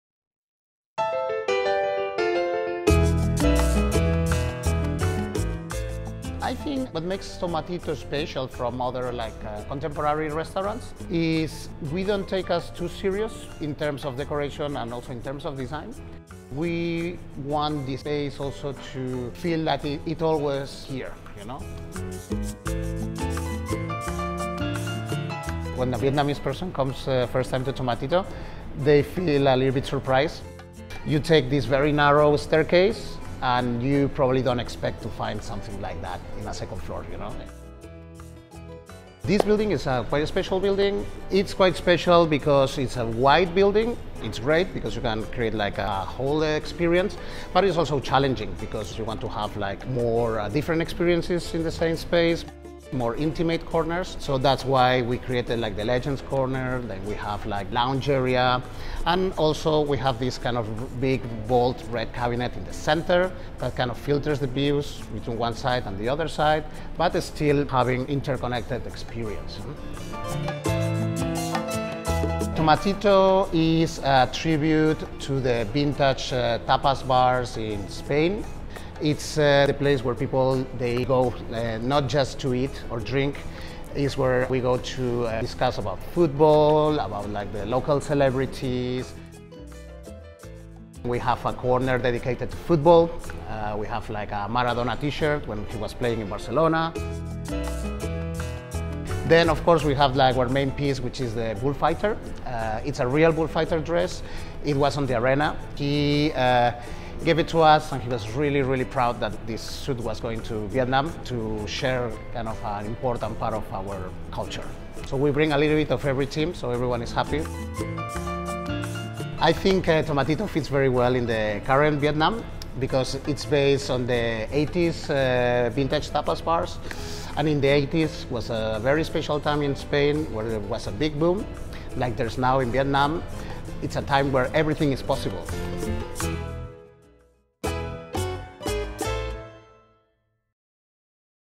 Tomatito Saigon: A Tribute To Spanish Vintage Tapas Bars | Vietcetera SPACES
Inspired by the aesthetics of vintage Spanish bars, Tomatito Saigon beautifully incorporates contemporary twists in both its interior design and decorations. Mark Trullas walks us through the story behind each design elements in the tapas bar.